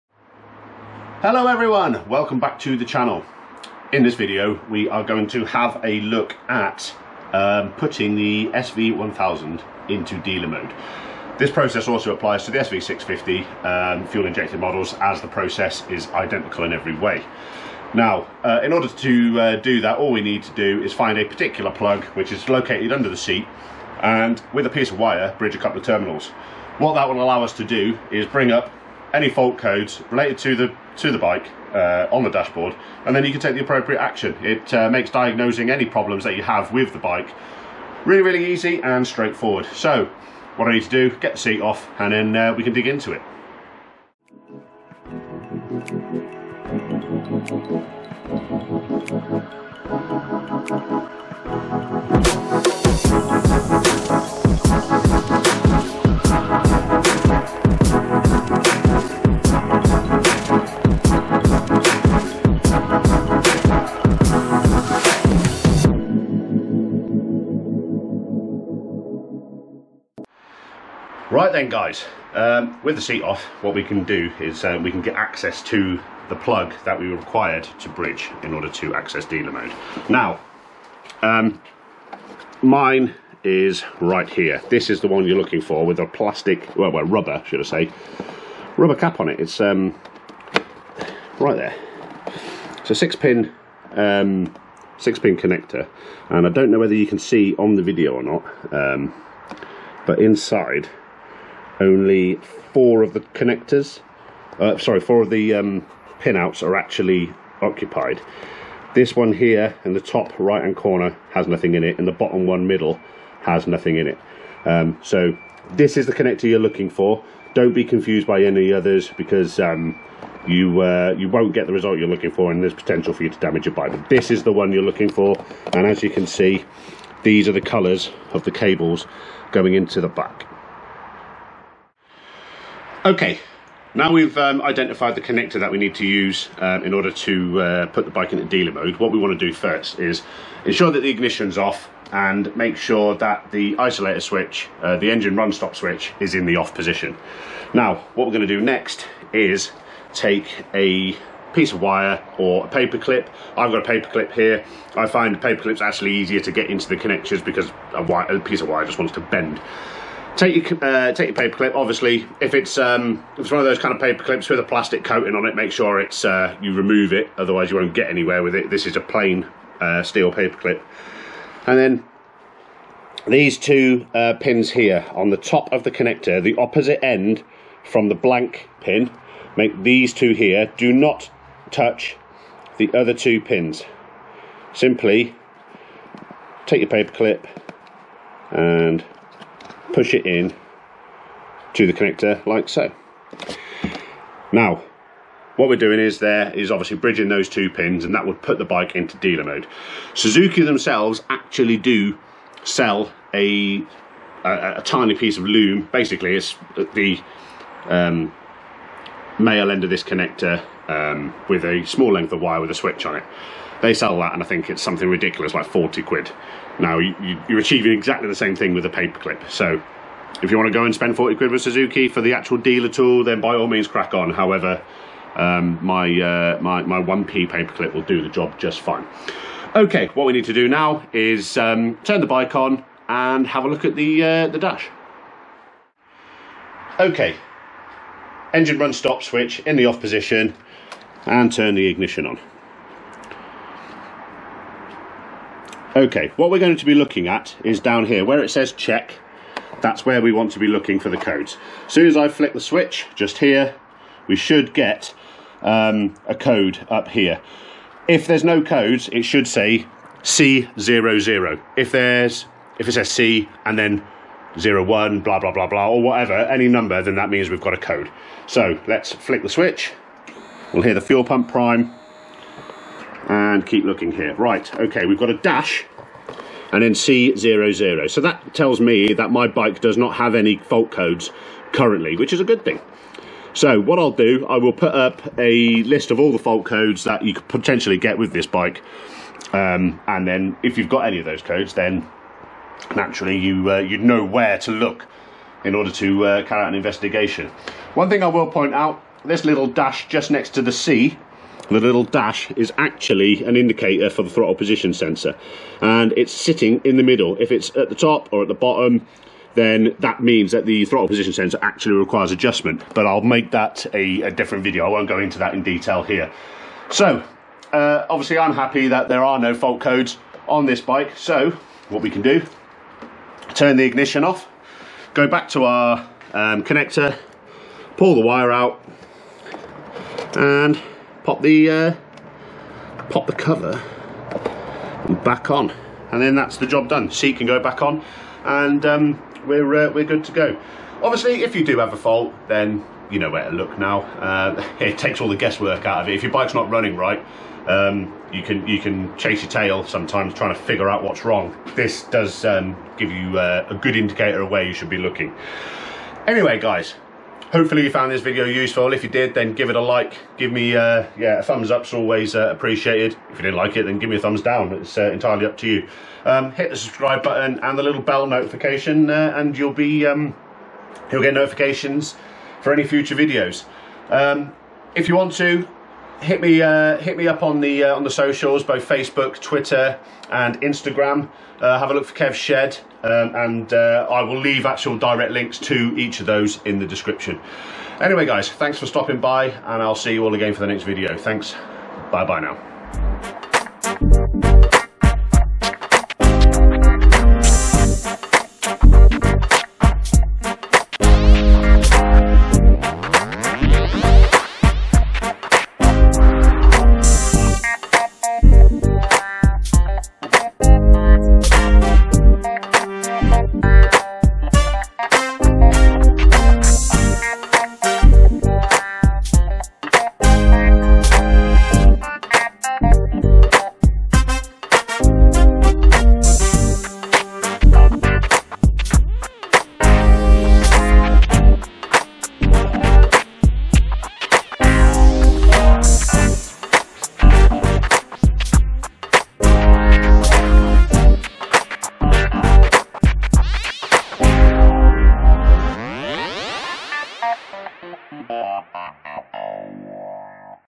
Putting The Suzuki SV1000 & SV650 Into Dealer Mode And Reading Diagnostic Fault Codes
Hi everyone, welcome back to the Shed!

In this video I am going to demonstrate how to put your Second Generation (Fuel Injected) Suzuki SV1000 or SV650 in dealer mode.

Dealer mode will allow you to read off any diagnostic fault codes that the bike may be displaying an FI light in reaction to.

This should save you considerable time and money over taking your bike to a Suzuki dealer or specialist.

If you wish to offer support to my channel and enable me to continue to bring you quality content then please check out my Patreon page at the following - Thank you.

Join me on the socials at the following links